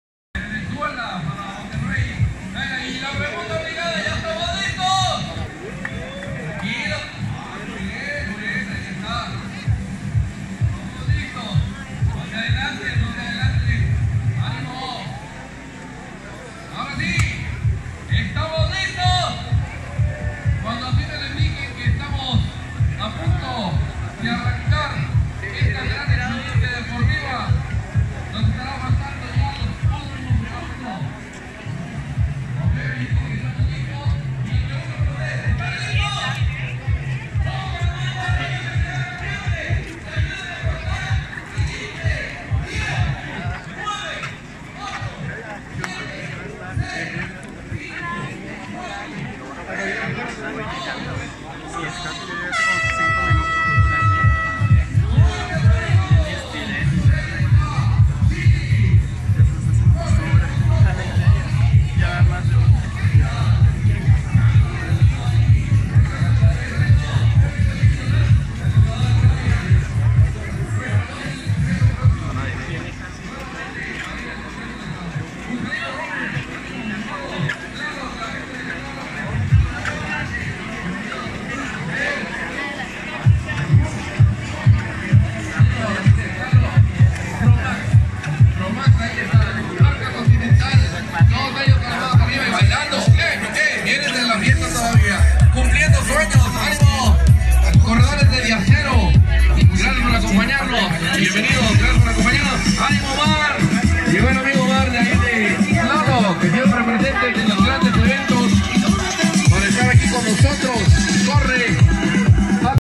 【メキシコ】マラソン大会 緊張のスタート！
Buenos días　皆さんこんにちは！メキシコのブルース鈴木です！
この番組では駐在しているメキシコの日常を動画でお伝えします。

マラソン大会10kmに参加してきましたの続きです。
スタート映像です！